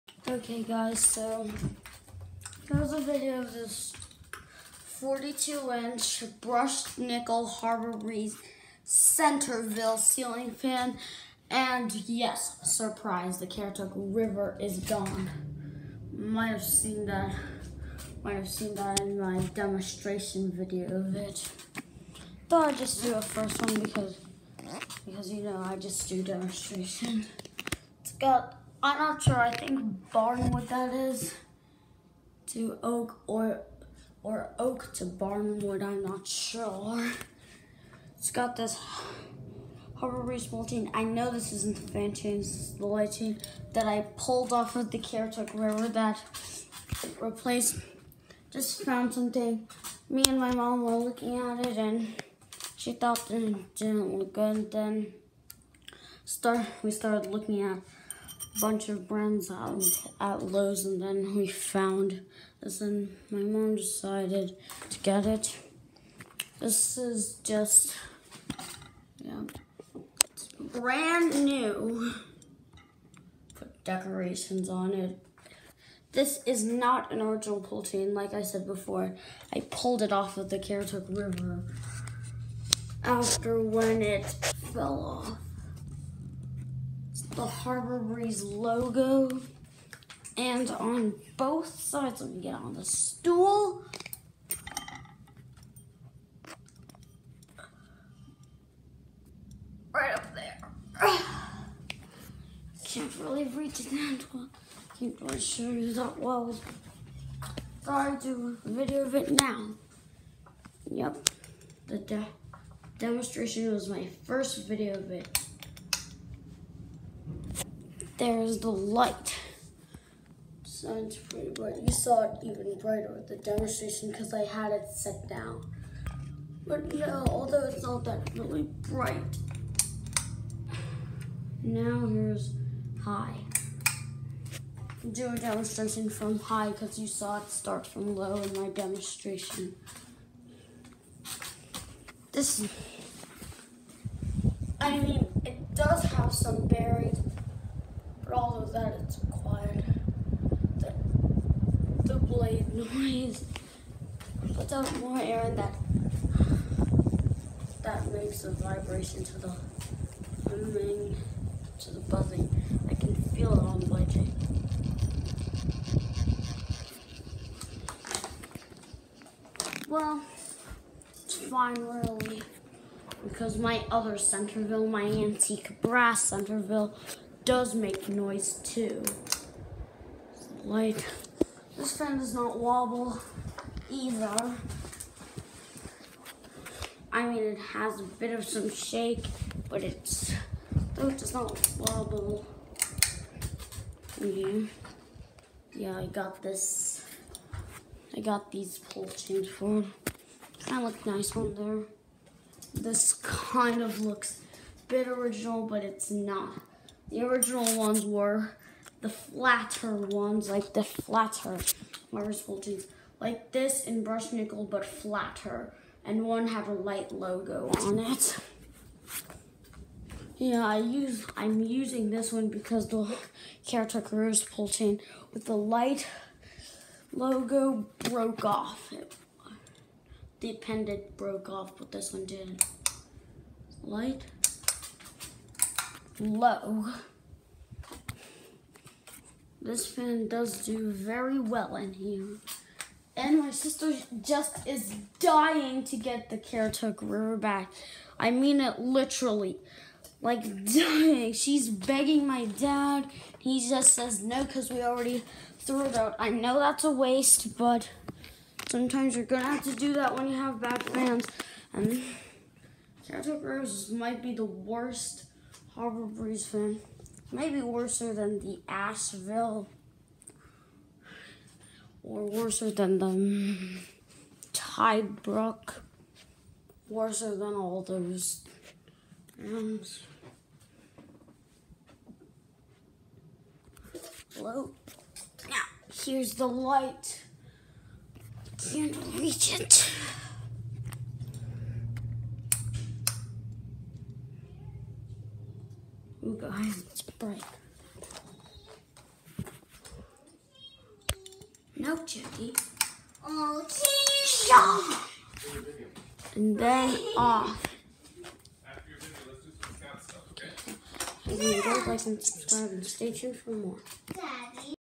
42” Harbor breeze Centreville (2 of 3)
I was going to replace the Caratuk river with a Barnstaple bay but because they are no longer for sale i replaced it with a Centreville